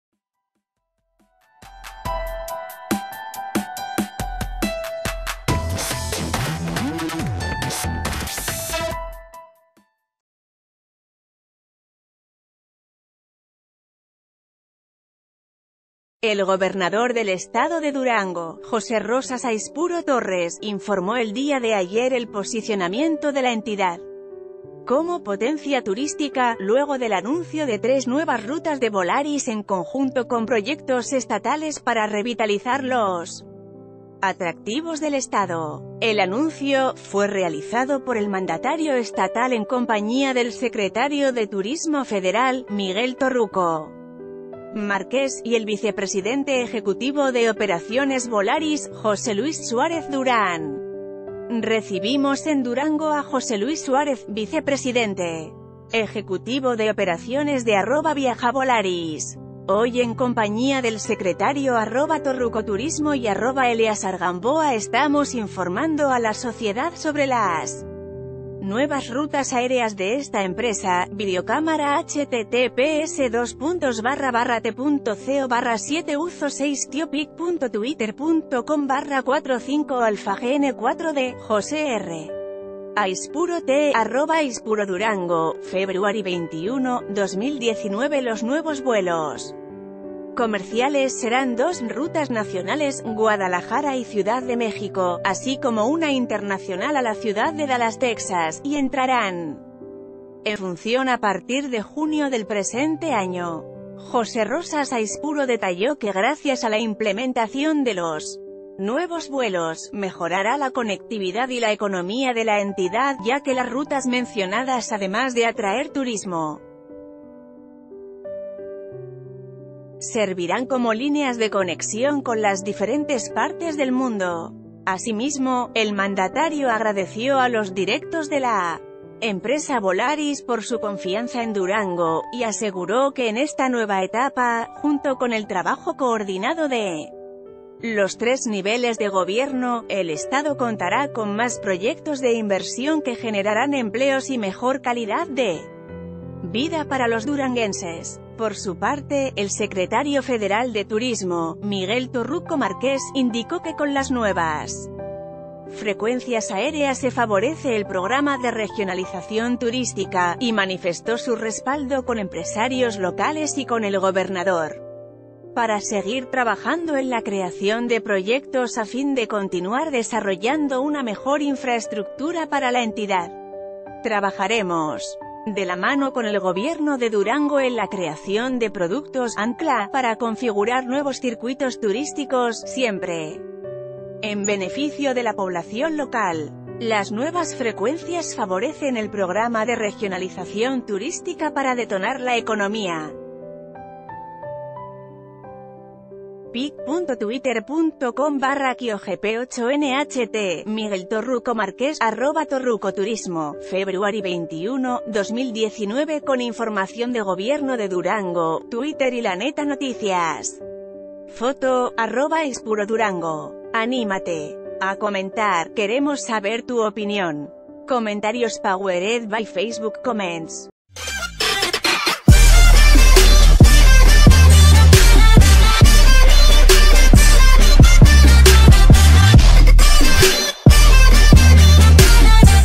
Volaris anuncia nuevos vuelos nacionales e internacionales para Durango
El gobernador del estado de Durango, José Rosas Aispuro Torres, informó el día de ayer el posicionamiento de la entidad como potencia turística, luego del anuncio de tres nuevas rutas de Volaris en conjunto con proyectos estatales para revitalizar los atractivos del estado. El anuncio fue realizado ...